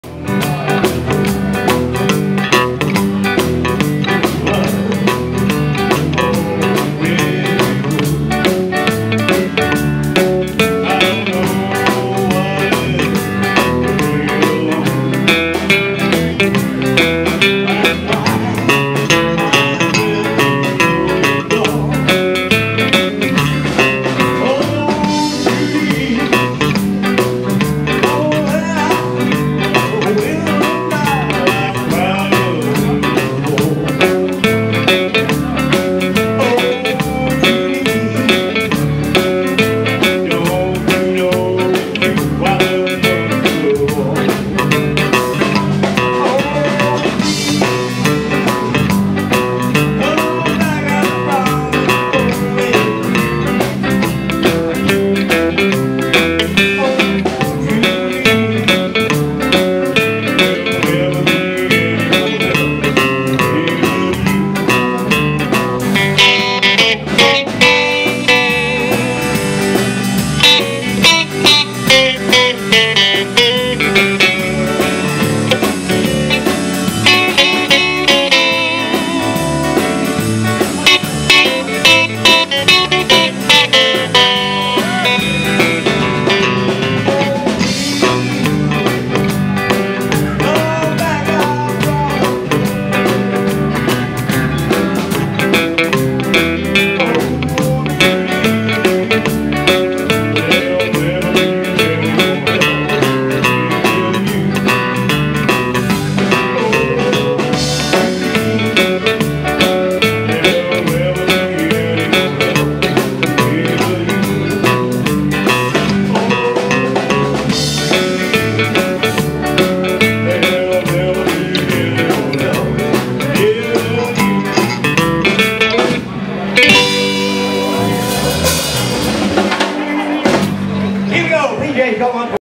THE ZODIACS BAND 8TH JAN 2016 OSBOURNE PARK BOWLING CLUB 6228
THE ZODIACS BAND  BIG FRIDAY NIGHT ROCKNROLL PARTY IN MEMORY OF THE KING ELVIS AND PRICILLA NIGHT 8TH JAN 2016